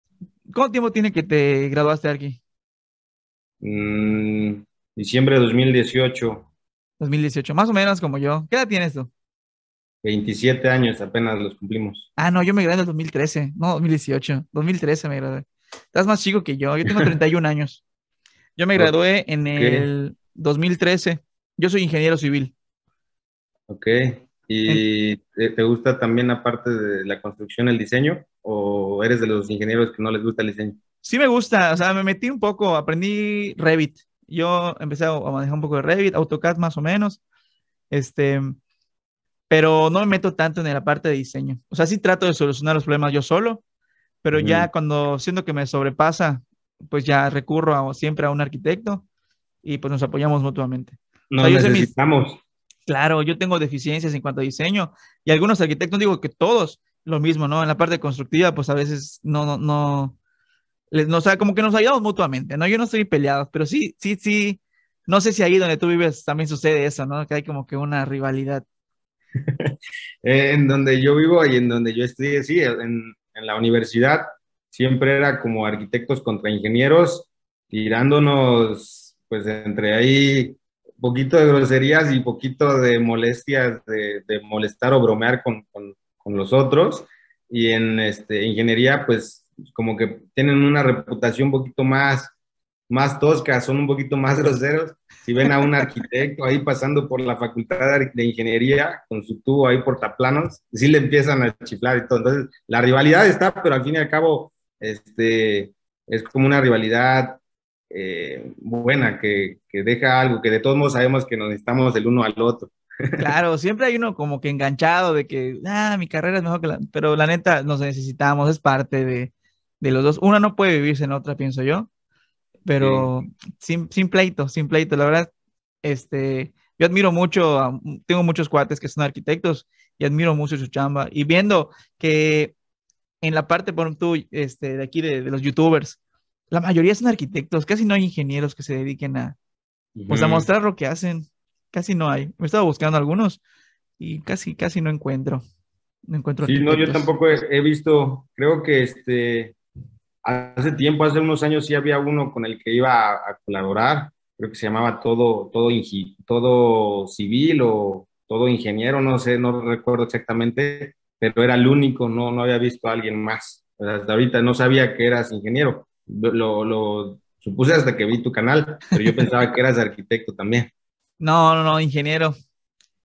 INGENIEROS VS ARQUITECTOS - ARTOSKETCH
EN ESTE VIDEO PLATICAMOS UN POCO DE LA RIVALIDAD QUE ALGUNOS COLEGAS LLEGAN A TENER